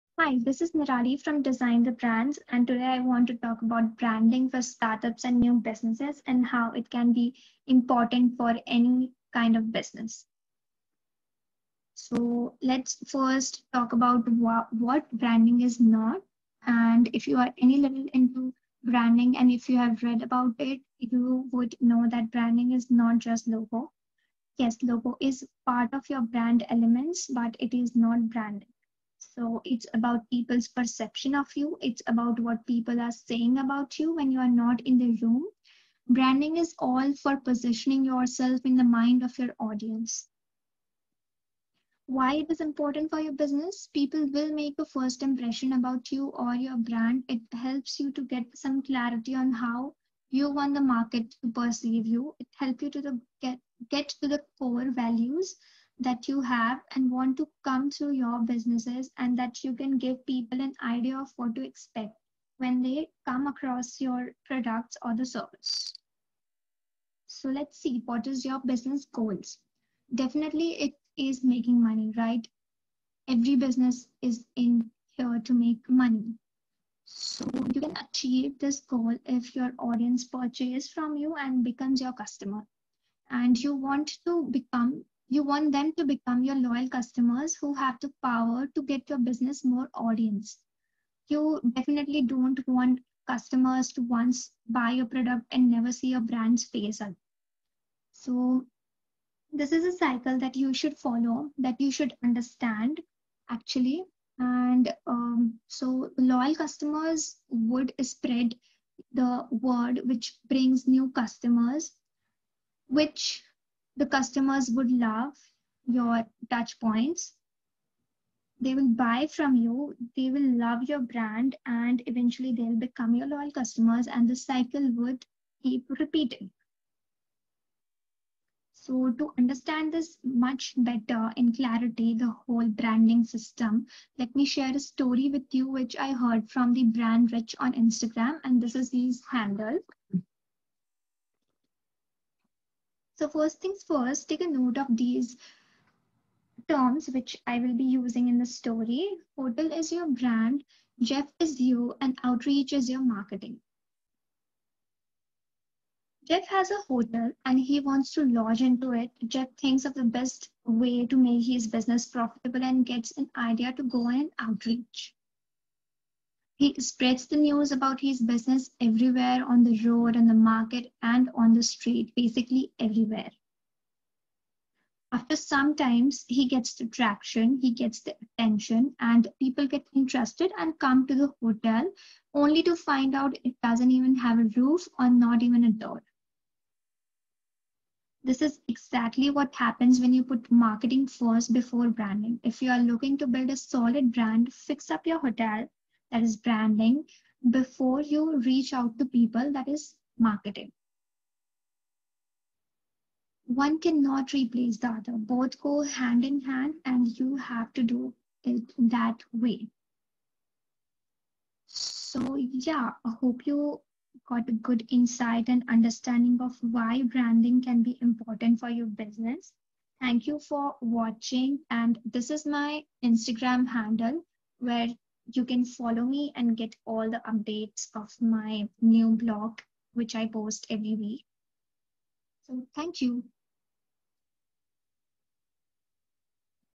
Is Branding important for startups & businesses?
The video will give you a better understanding of why branding can be important for your business to help you connect with your audience better and increases your Brand ROI.